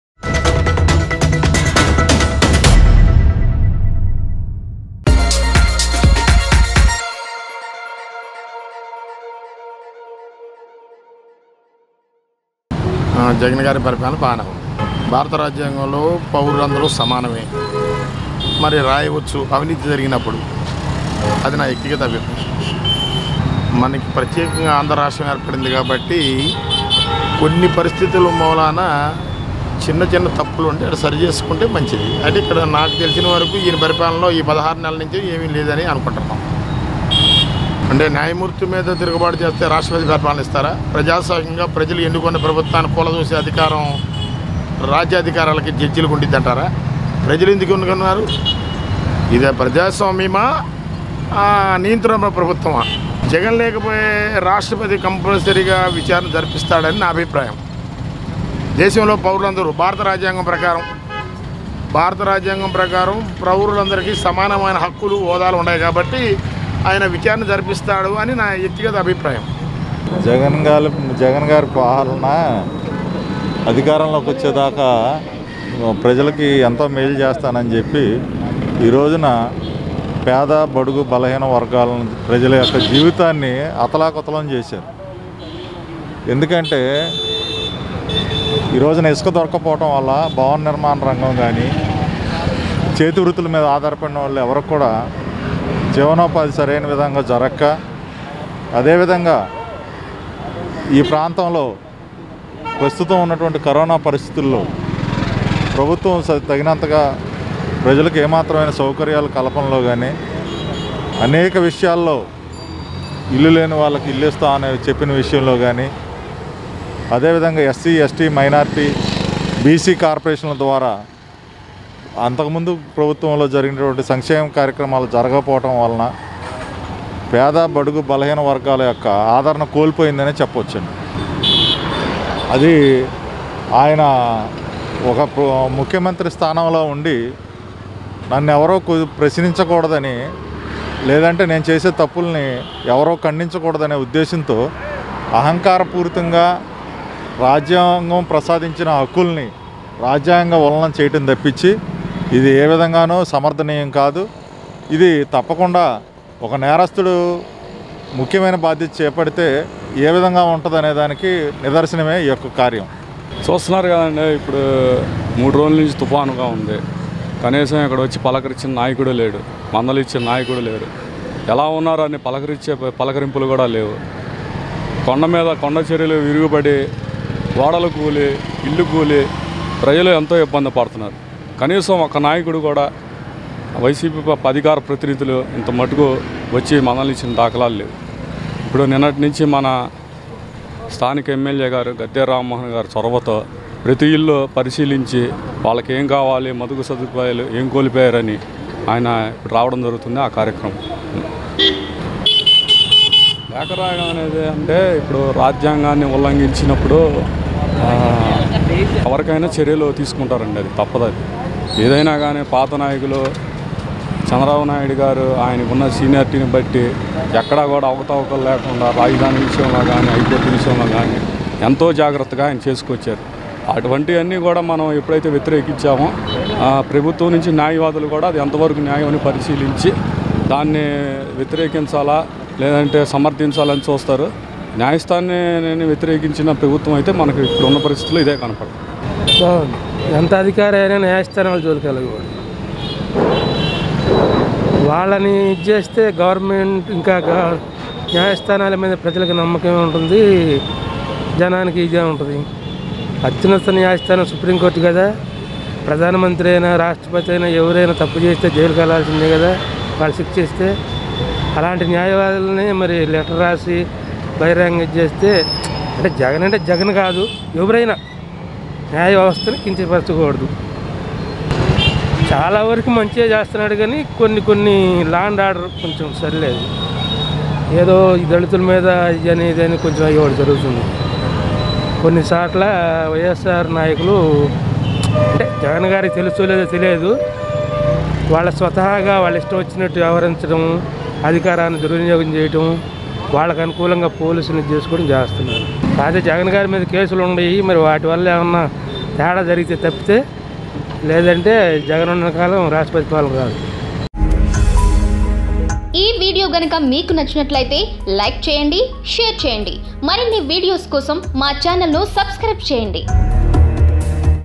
Public Reaction On AP Politics | CM Jagan Ruling Very Good | Public Talk | PDTV News
జగన్ లేఖపై విచారణ జరగుతుంది | Public Reaction On AP Politics | CM Jagan Ruling Very Good | Public Talk | PDTV News